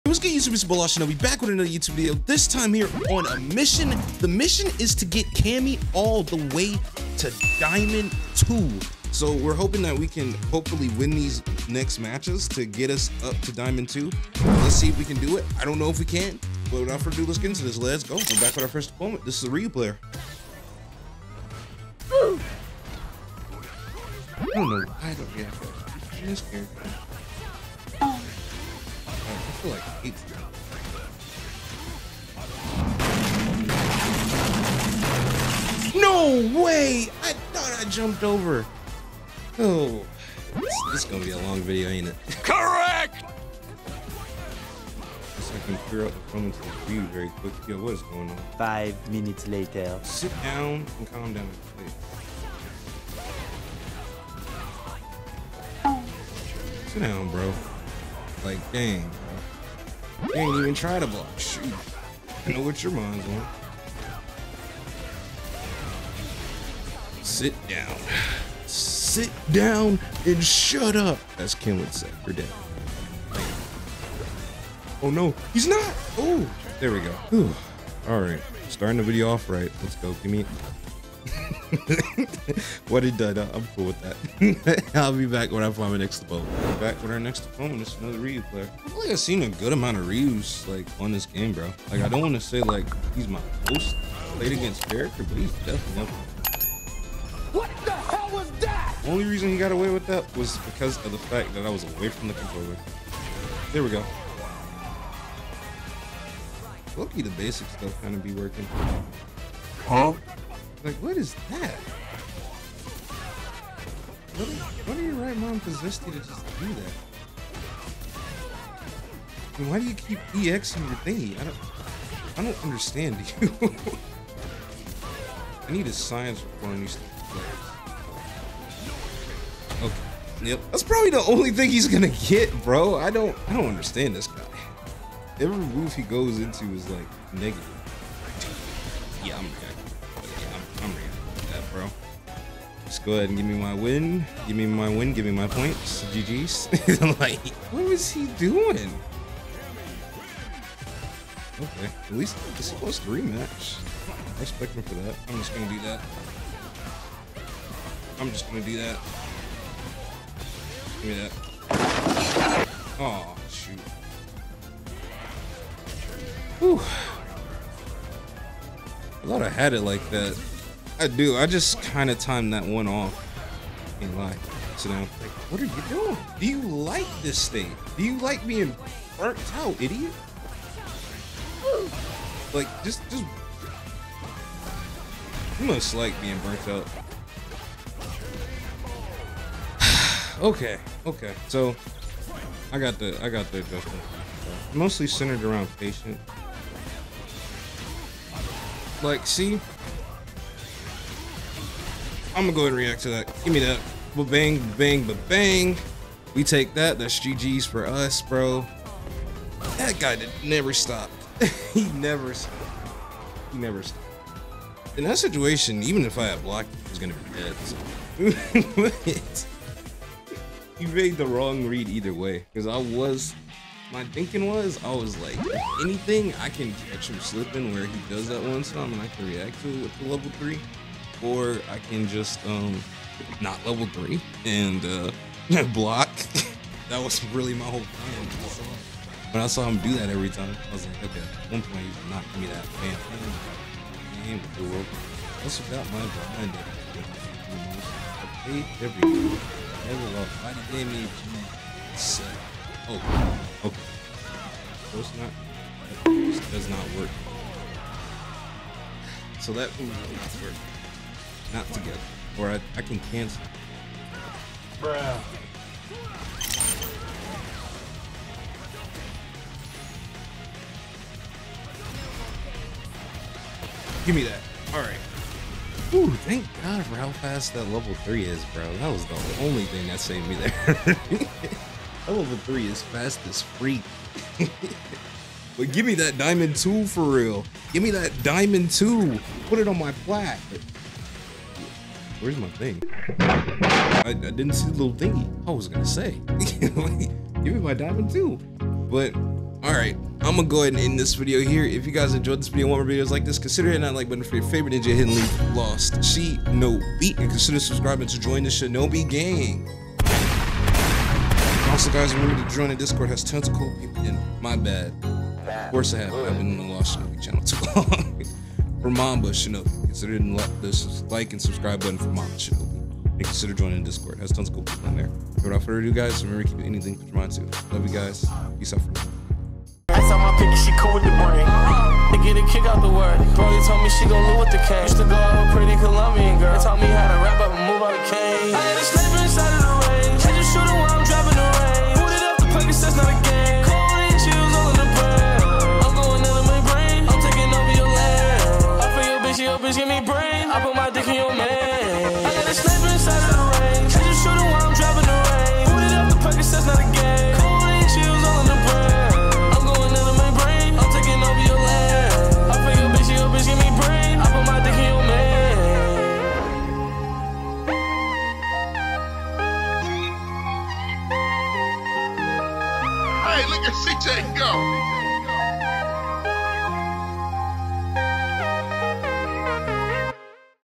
CAMMY IS THE NEW BEST IN THE WORLD!
In this video I will be playing as Cammy and seeing if I can get her to Diamond today. Will I do it I don't know but we are going to find out let's get into this.